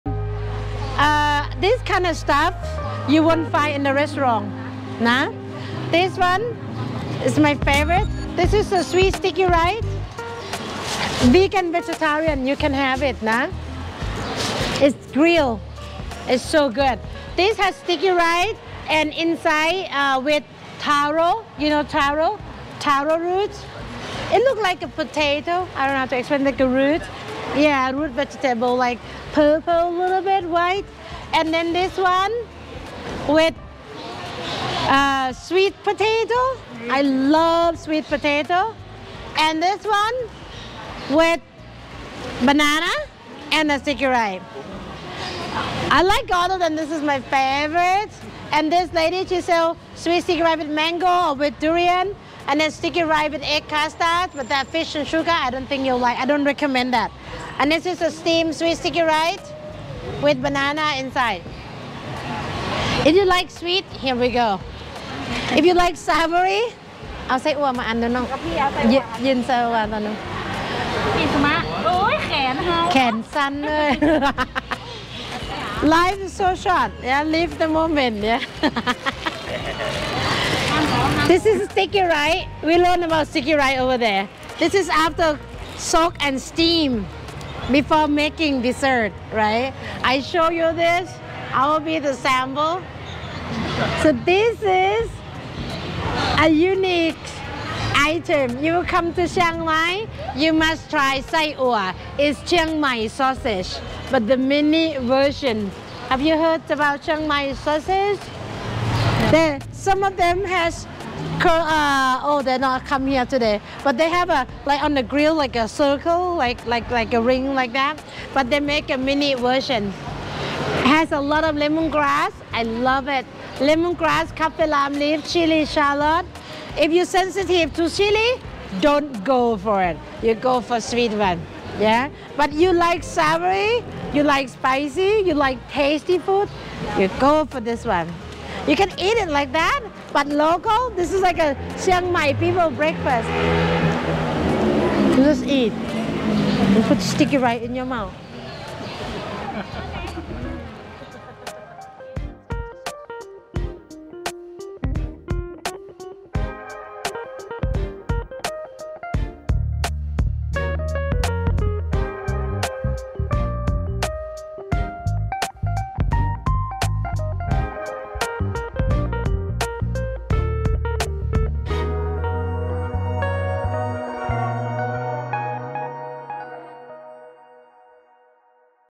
Thai Secret Cooking Class & Street Food Tour. 25 January 2023
Best Activity in Thailand 2023 A Thai cooking class with me at my school in Chiang Mai, Thailand “Thai Secret Cooking School.” Join my cooking class, walk and shop our local Thai market, continuing into my organic garden to pick some fresh ingredients, next off to the kitchen. Thai cooking, eating, and napping fun! Thai Secret Cooking School & Organic Garden in the country side of Chiang Mai, Thailand. We have cooked Mountains of Pad Thai! Oceans of Thai Curry! Jungles of Papaya Salad! & Yottabytes of Media! Join our cooking class and have a fun day cooking in my open-air kitchen. Learn some secrets of Thai cooking and bring home new Thai culinary skills a unique and personalized souvenir. “Do you know my secret yet?”